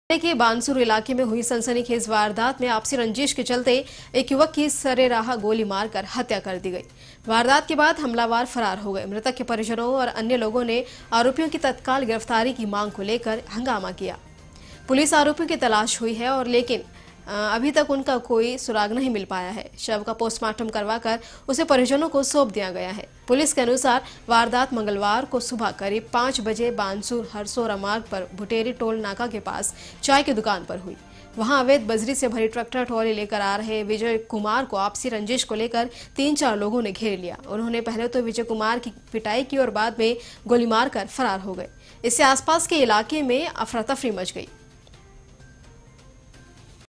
अलवर में आपसी रंजिश में युवक को घेरकर गोली मारी, मौत पर मचा बवाल
1-Dausa घर के बाहर खड़े नाबालिग की गोली मारकर हत्या, आरोपी गिरफ्तार
2-Jhunjhunu में युवक की बेरहमी से पिटाई, सोशल मीडिया पर वायरल हुआ वीडियो
3-Udiapur में हुआ भीषण सड़क हादसा: ट्रक में घुसी बेकाबू कार, बच्चे समेत पांच लोगों की मौत
4-Kota : आधी रात को दो दुकानों में लगी भीषण आग में लाखों का नुकसान, शॉर्ट सर्किट की आशंका
5-Sikar में पटाखों पर हुआ विवाद: युवक ने साथियों के साथ घर में घुसकर ममेरे भाई को गोली मारी
6-Alwar में आपसी रंजिश में युवक को घेरकर गोली मारी, मौत पर मचा बवाल
7-Jhalawara विवाहिता ने कुएं में कूदकर दी जान, ससुराल पक्ष पर लगा हत्या का आरोप
8-Tonk- एक ही गांव के 4 नाबालिगों ने 6 महीने में तोड़े 1 दर्जन ATM, लेकिन रुपए नहीं चुरा पाए
9-Ajmer- गांधी चौक पर लगी राष्ट्रपिता महात्मा गांधी की प्रतिमा खंडित की गई
10- Bikaner- राजस्थान के मेडिकल कॉलेजों में 960 पीजी की सीटें बढ़ेंगी